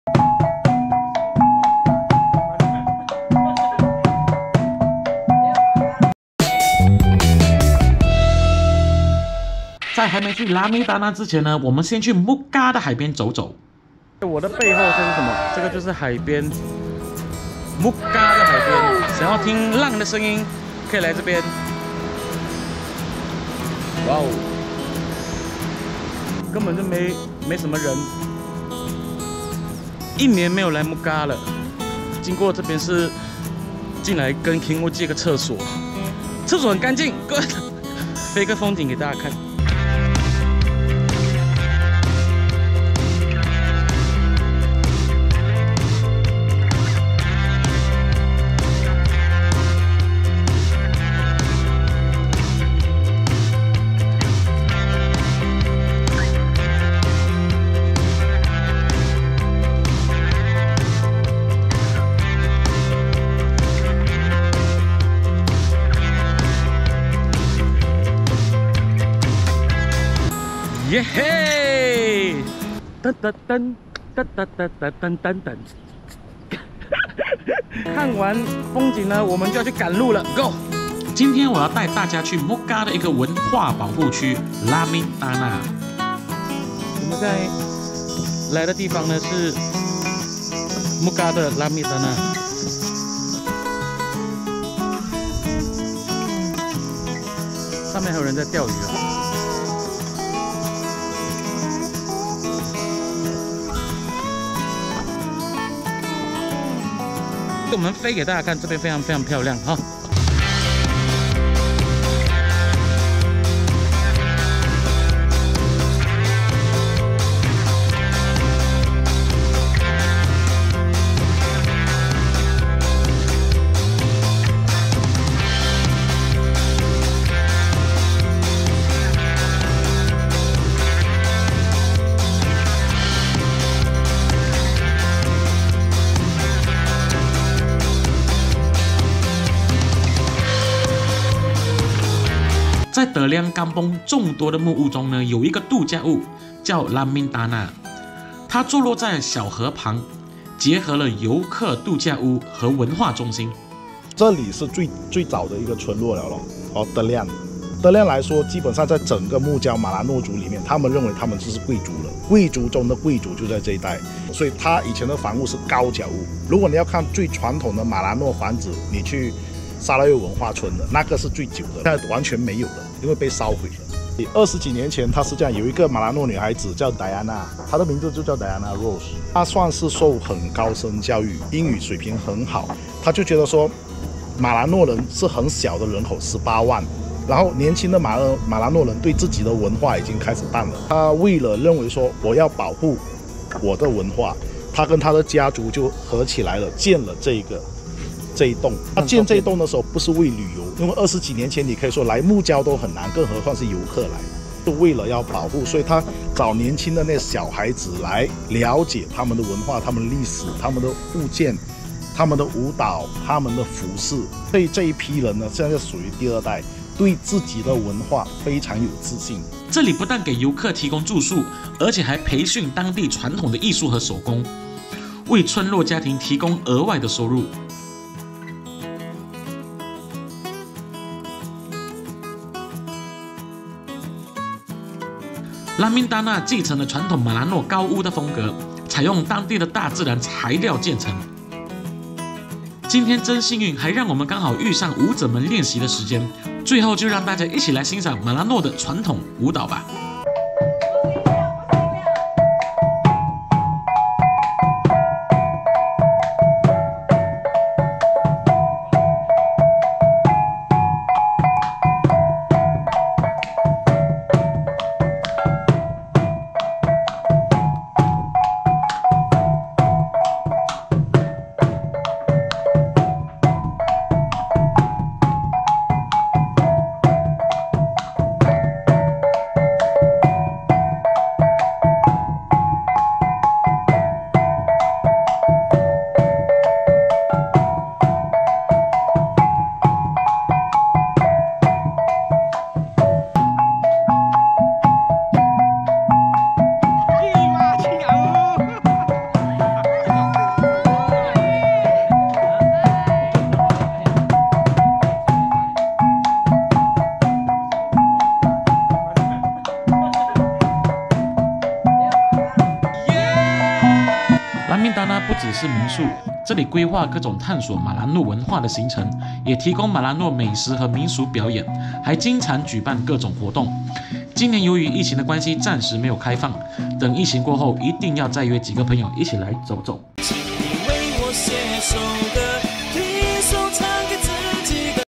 First Try Melanau Dance? Lamin Dana Tellian Mukah! 沐胶拉敏达娜! 砂拉越文化保护区
很榮幸去年有機會可以到沐胶的德蓮
拉敏达娜不止是民宿，这里规划各种探索马兰诺文化的行程，也提供马兰诺美食和民俗表演，还经常举办各种活动
今年由於疫情的關係暫時沒有開放，等疫情過後一定要再約幾個朋友一起來走走

◆喜歡我的影片，記得幫我【分享】和【按贊】
◆ 記得來關注陳叮咚喔！😍